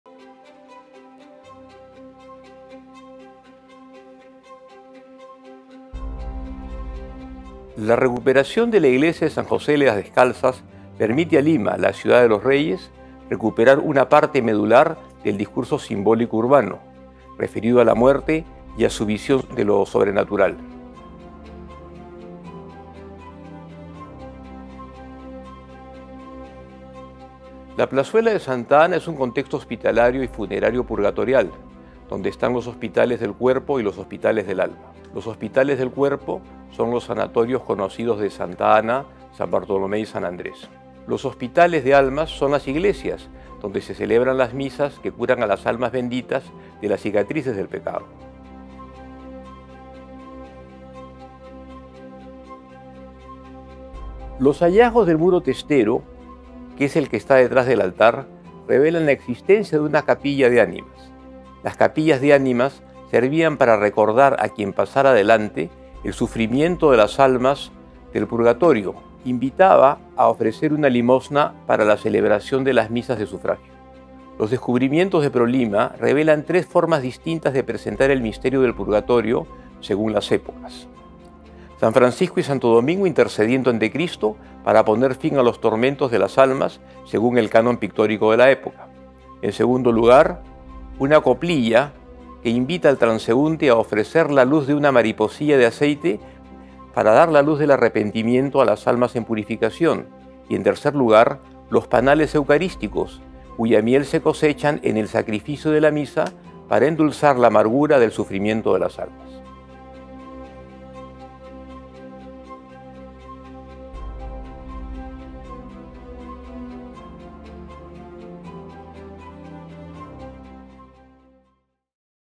San José de las Descalzas Church: The Secrets of its Mural Paintings
The historian José Gálvez reflects on the recovery of the front of the emblematic temple in Barrios Altos. During the works, carried out by Prolima, traces of ancient mural painting were found, analyzed by the specialist.